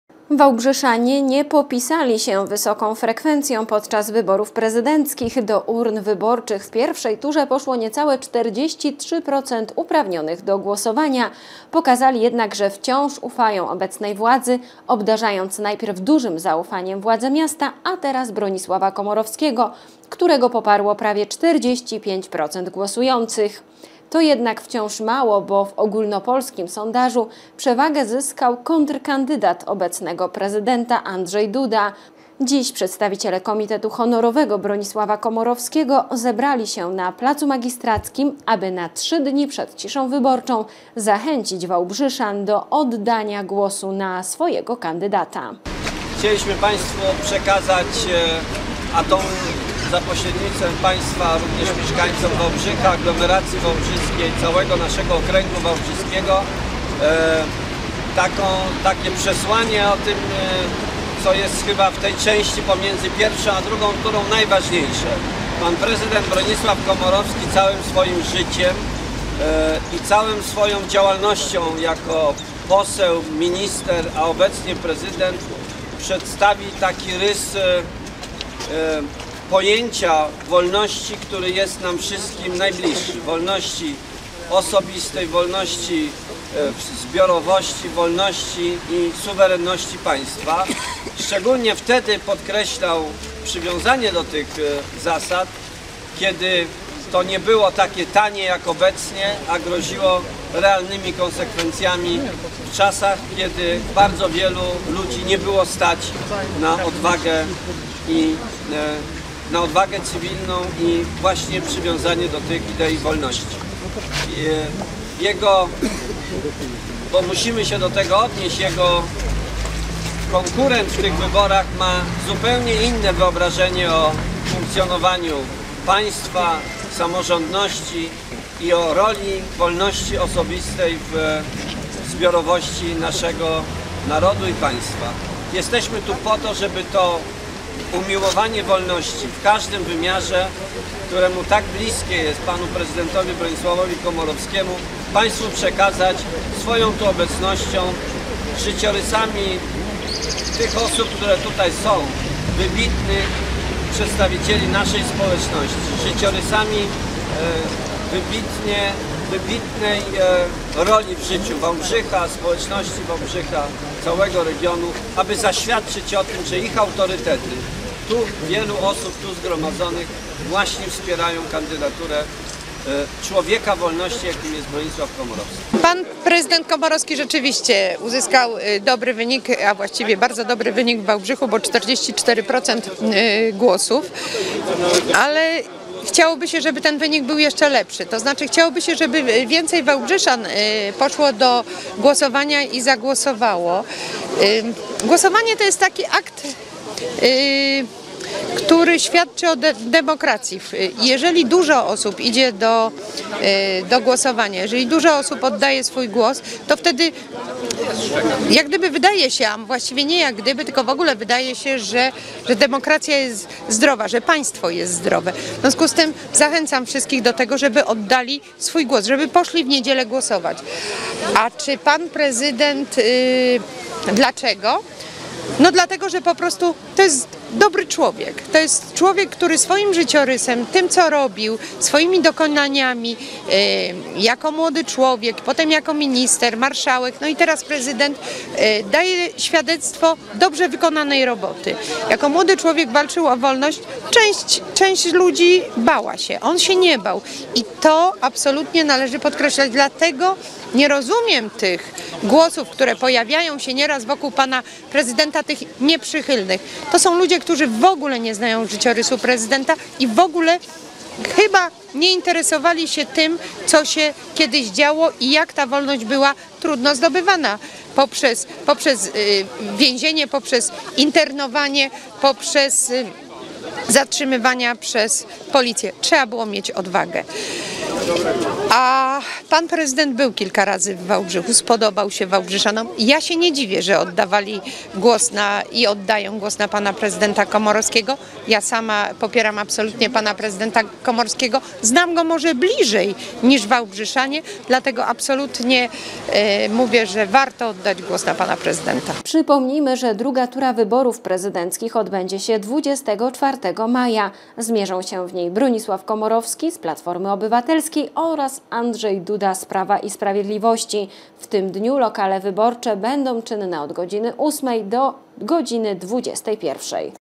Ostatnia prosta przed II turą
Działacze Honorowego Komitetu Poparcia Bronisława Komorowskiego zachęcali wałbrzyszan do głoswania na kandydata PO